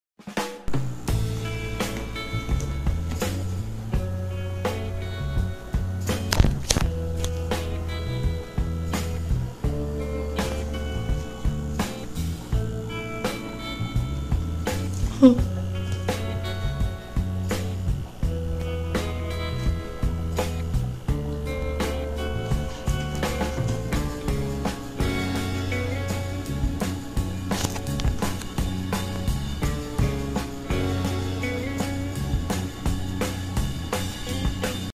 飼っている金魚がついてくるー
私が飼っている金魚の水槽を通ると必ずついてくるんです!!ちなみに金魚の名前は赤丸です!!